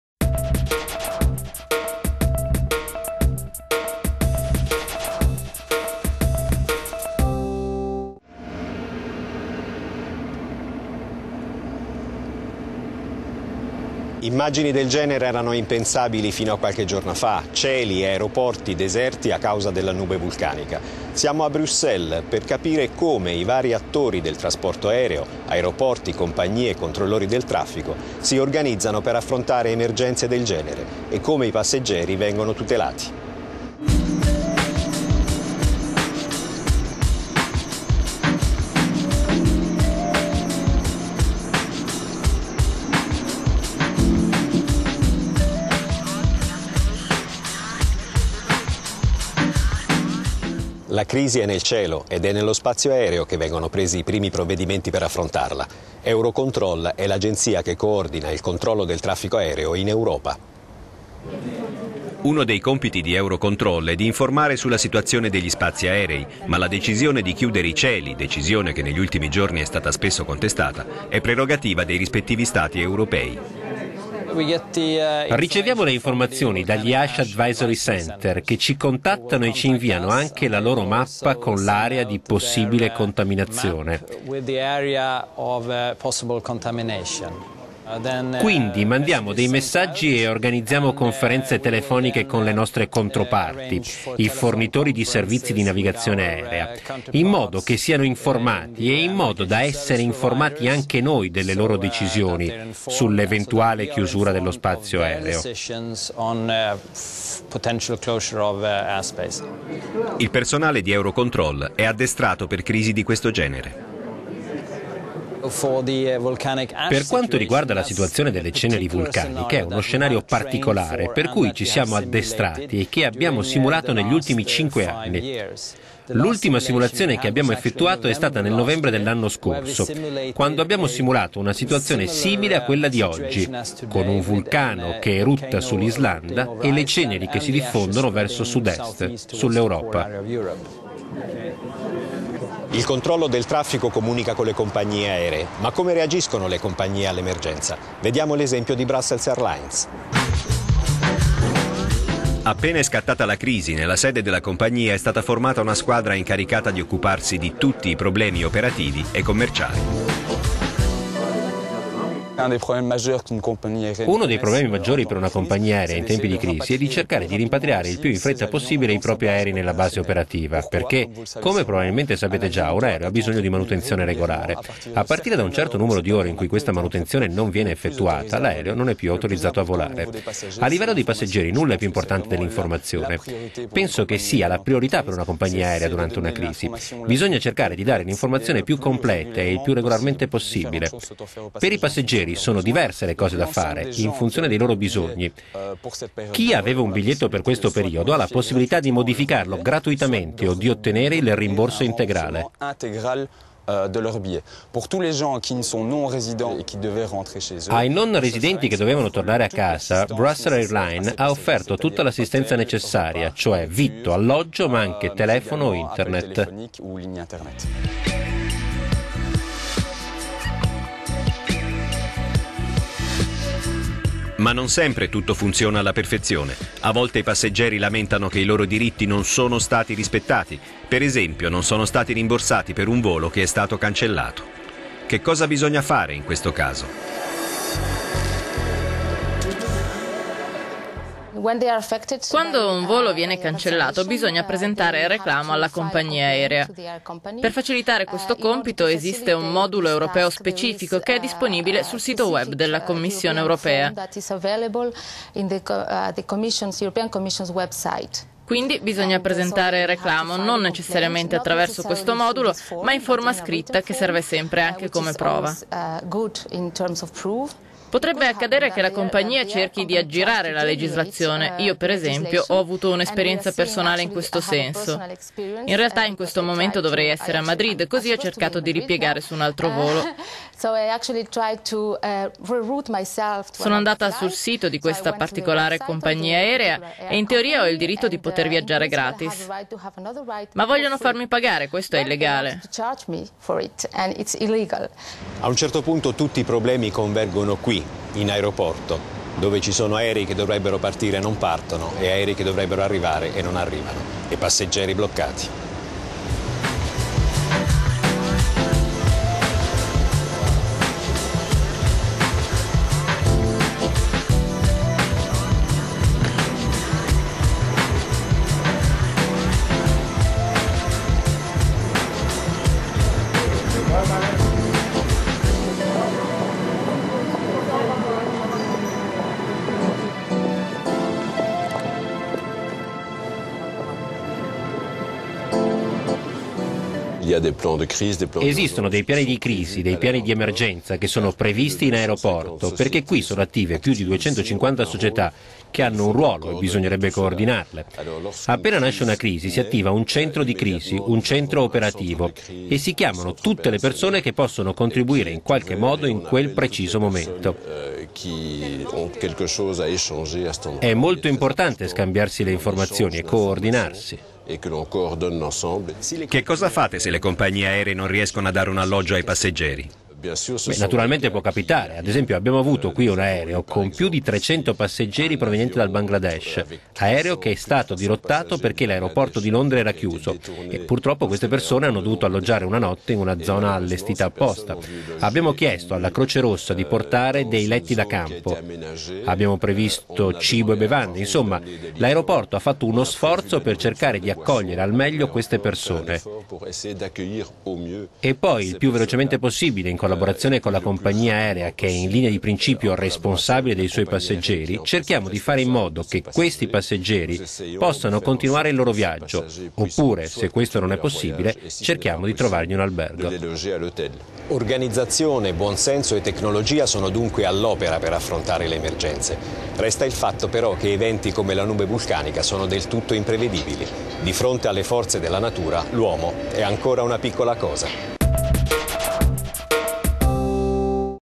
Caos nei cieli: retroscena e rimedi
Un evento senza precedenti per il sistema di trasporto europeo. Spazio aereo chiuso a causa di una nube vulcanica. Ecco come gli aeroporti, le compagnie aeree, il sistema di controllo del traffico sono organizzati per gestire emergenze del genere. E come i passeggeri vengono tutelati.